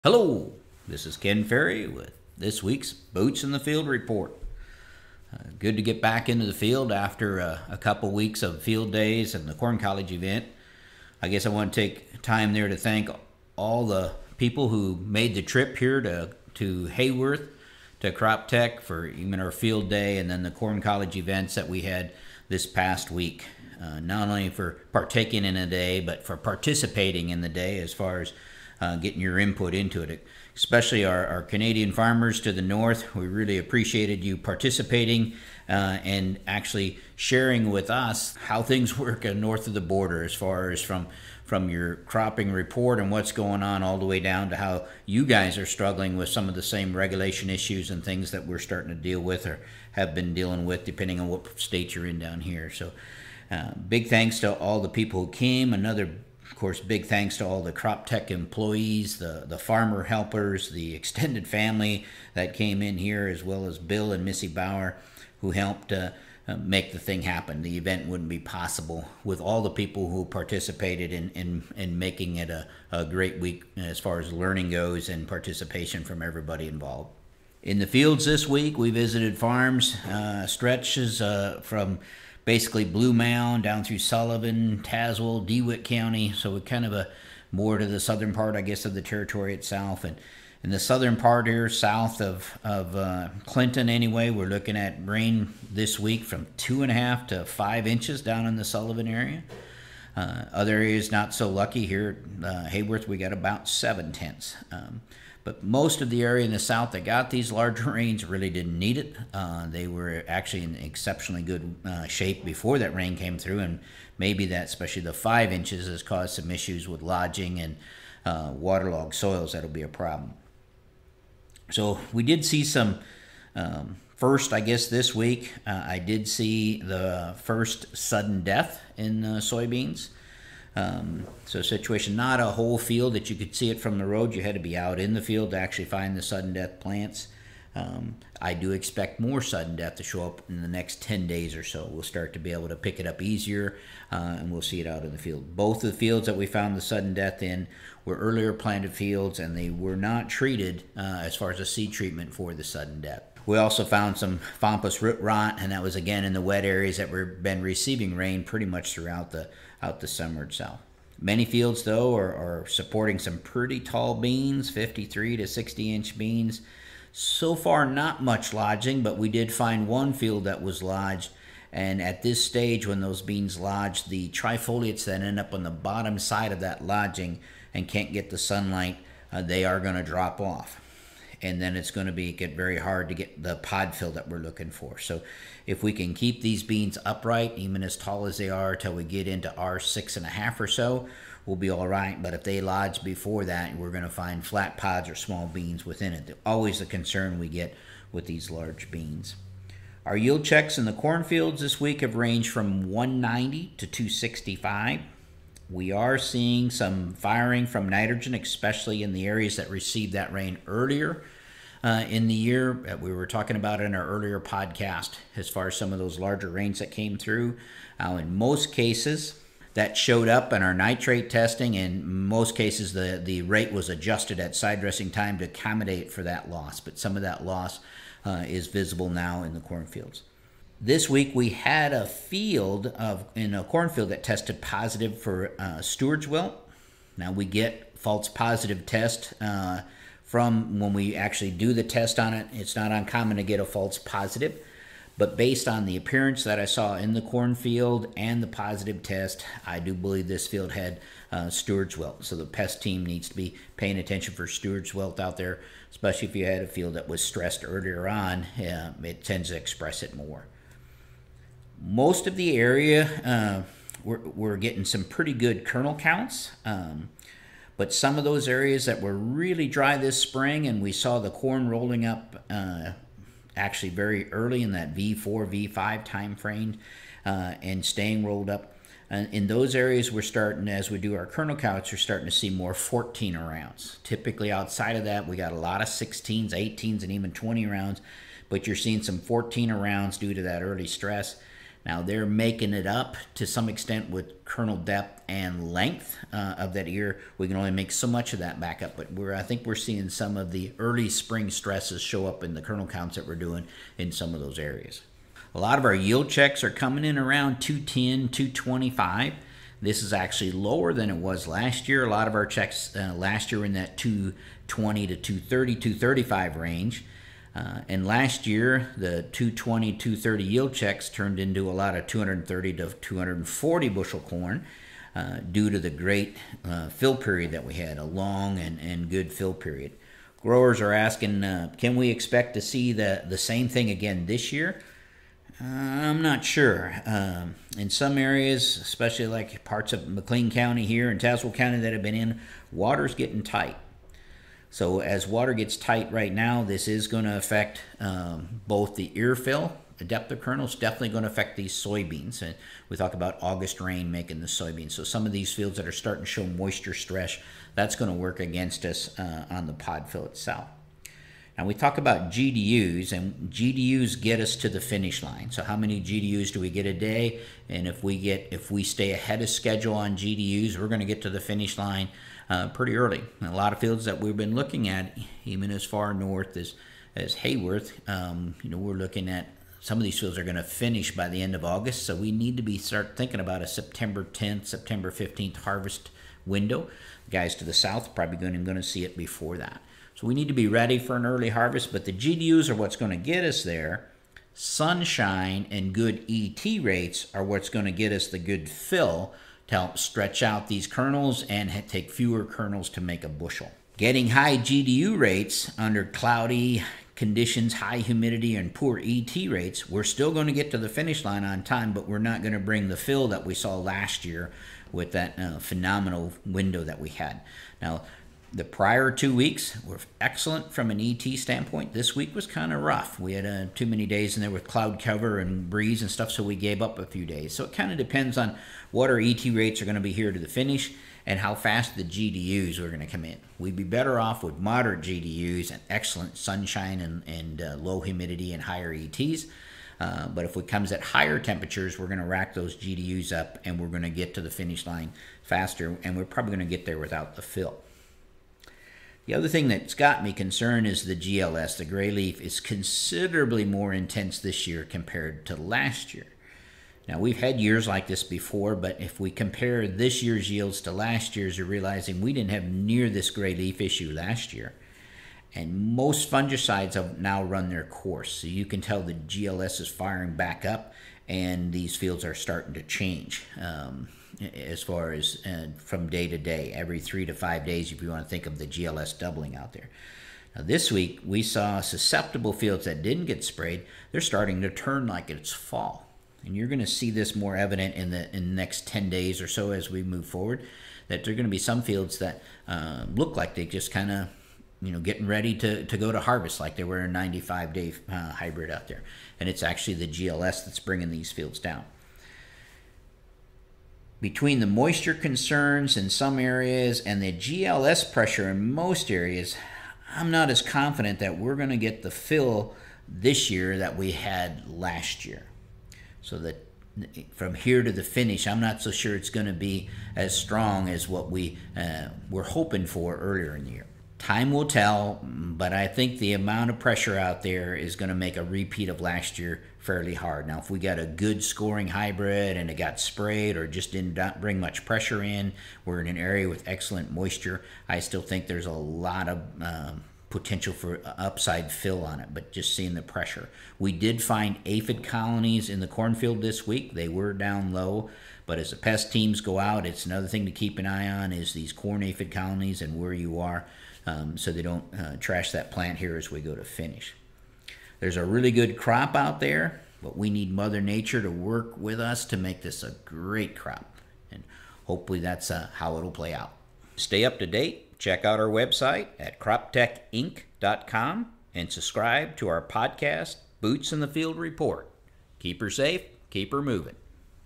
Boots In The Field Report for August 6, 2018
In this week’s Boots In The Field Report Ken shares that he has spotted: sudden death, pompous root rot, Stewart’s wilt, aphid colonies in the corn, and gray leaf spot. He talks about his concern of what could take the top off this corn crop and what we need to finish out the season strong.